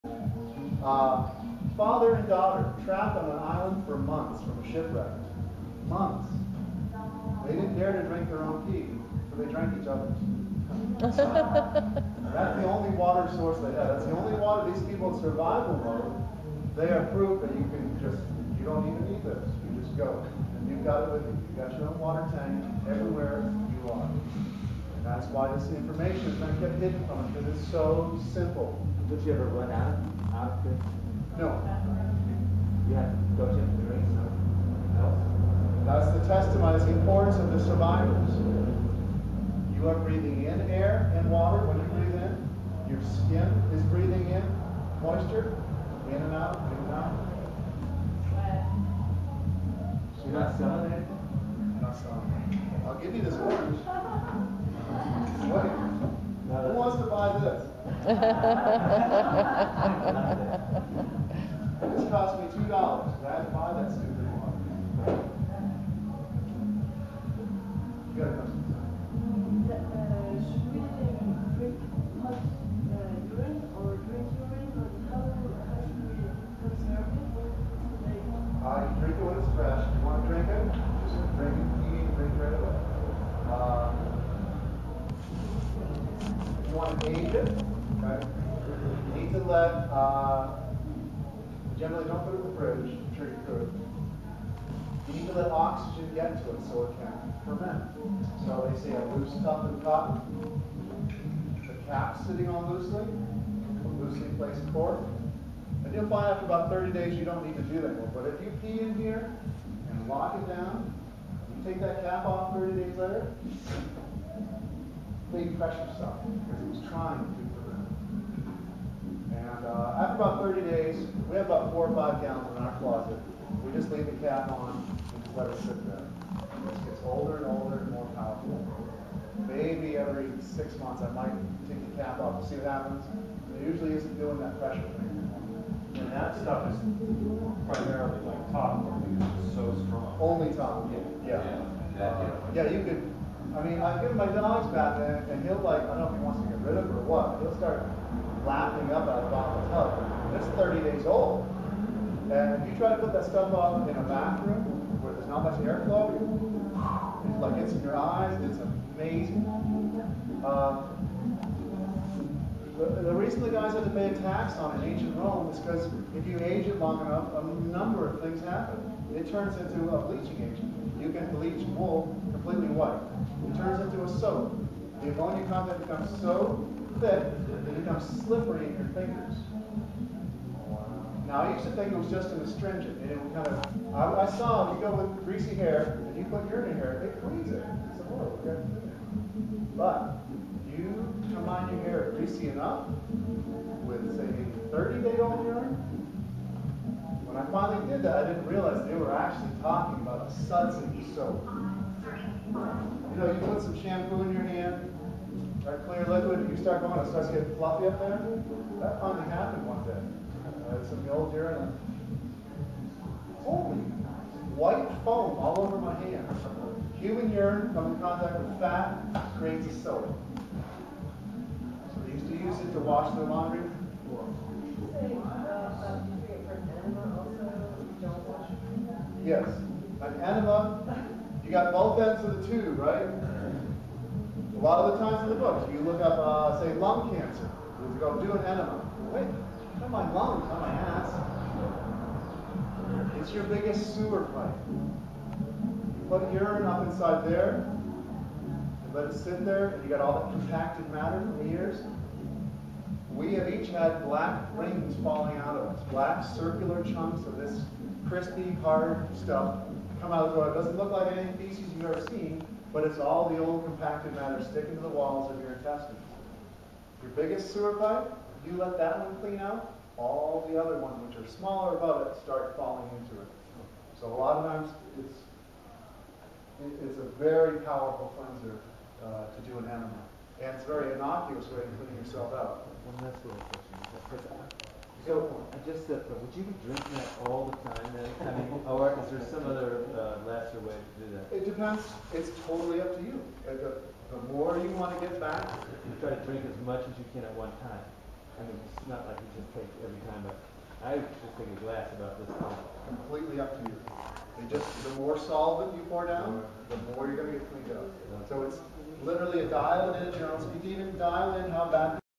Andrew Norton Webber: Distilled Liquids and their Effect on Organic Creatures 2g
Andrew Norton Webber, artist --presenting his lecture on Distilled Liquids and their Effect on Organic Creatures in the Conference Center of Resort de Agua del Hierro (RAH) in the pristine sacred valley of Vilcabamba in Southern Ecuador.